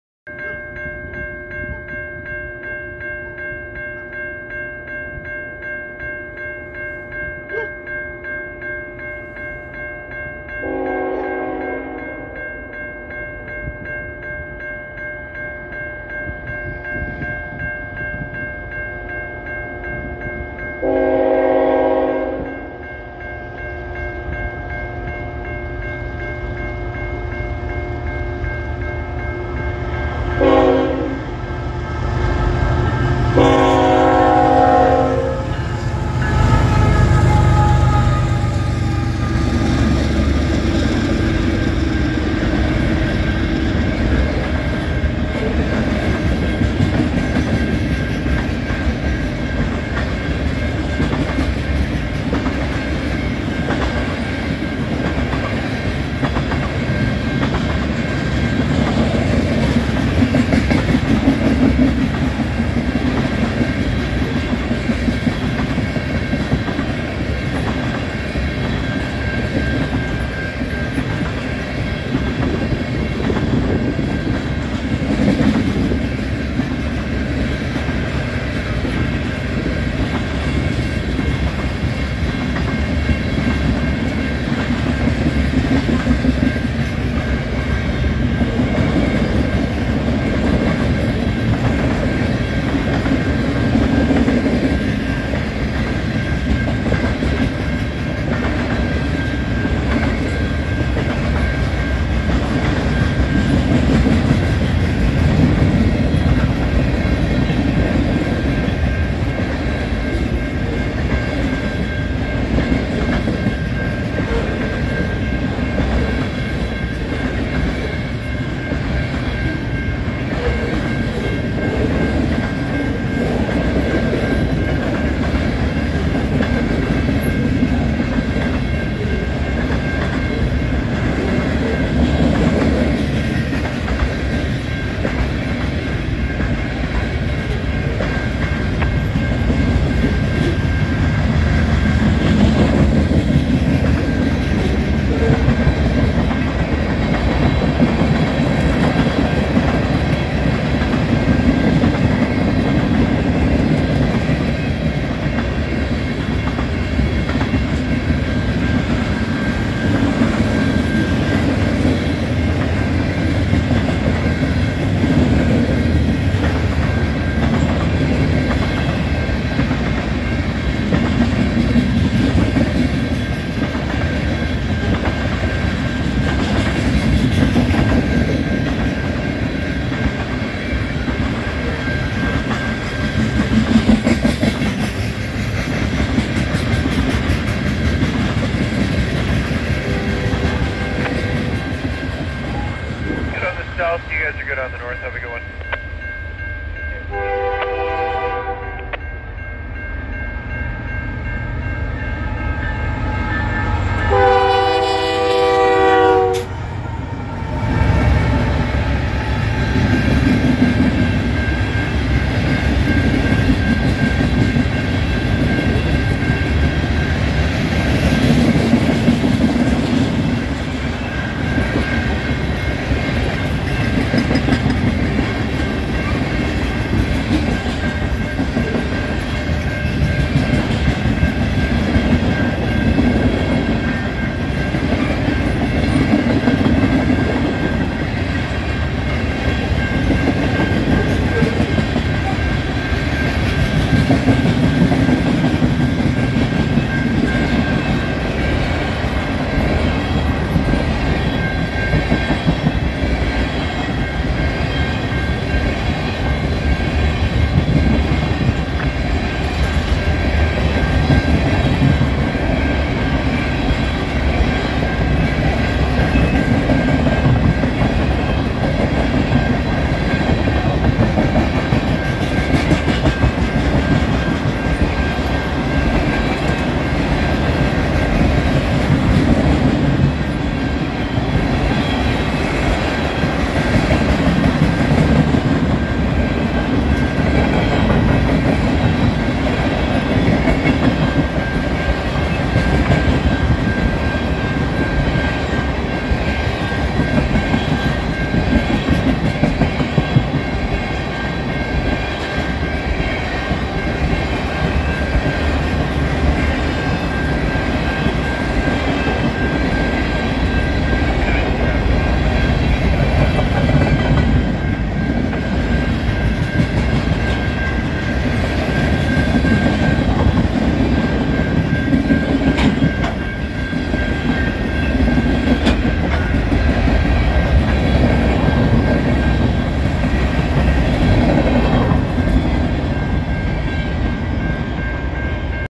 (Train Meet/Ex-CitiRail/AC446CM Dash9) CN X302 - (CN100) - 3218 & 3926 And CN 397- 3301 & BNSF 6950
Train #1 (X302) - Pulling 120 Cars

Train #2 (397) - Pulling 112 Cars

640 Axels

Speed: 36

Recorded in Lynden Ontario

Quite the Power on both Trains! Wasn't able to fit alot in the Title